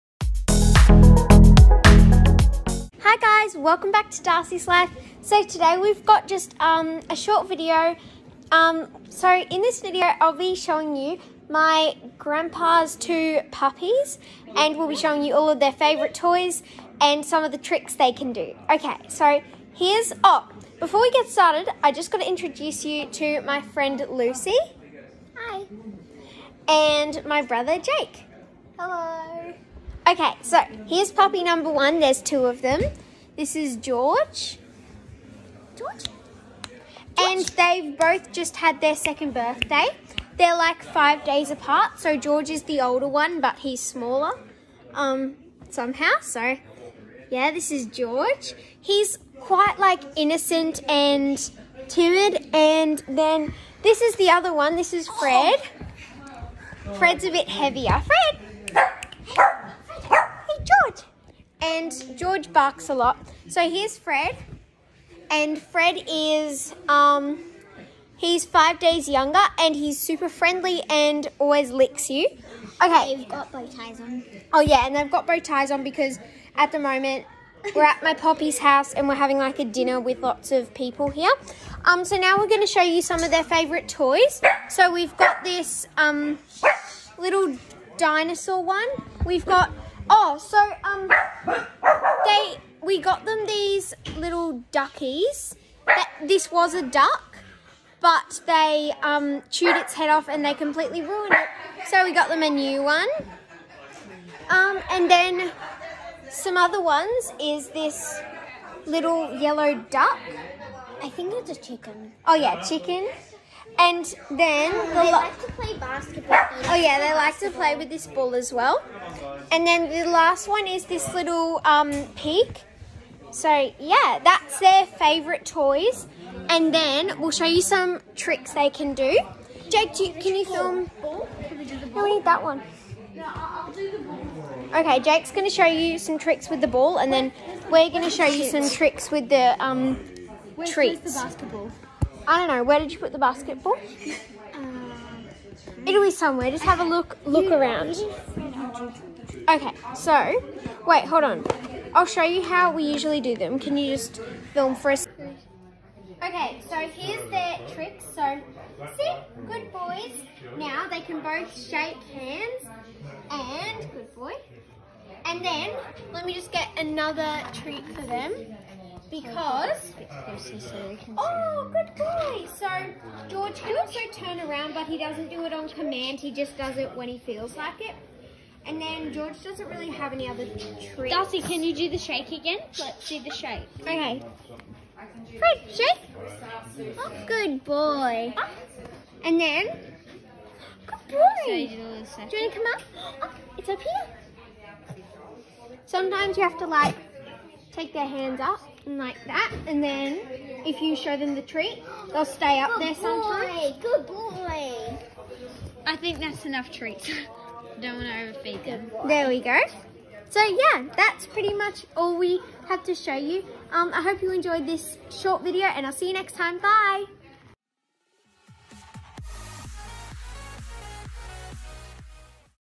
Puppies!!!
Meet my Grandpas Shih Tzu Puppies!!!